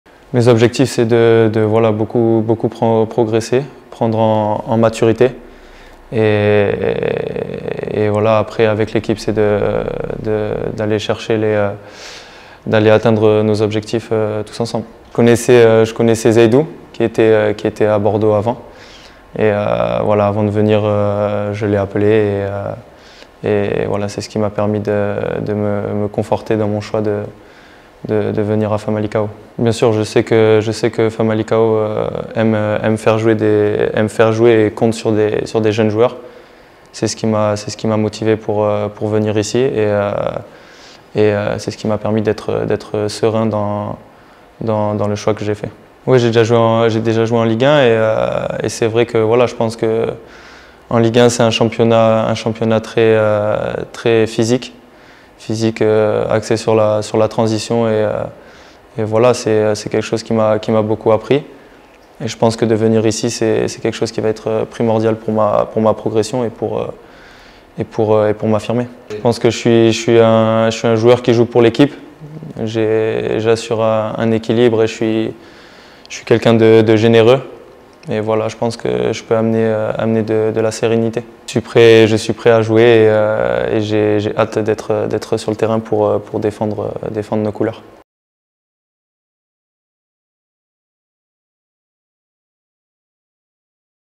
Tom Lacoux é reforço do FC Famalicão
O Futebol Clube de Famalicão – Futebol SAD anuncia ter chegado a acordo com o FC Girondins de Bordéus para a transferência do jogador Tom Lacoux. O médio francês, de 21 anos, rubricou um contrato válido até ao final da presente época.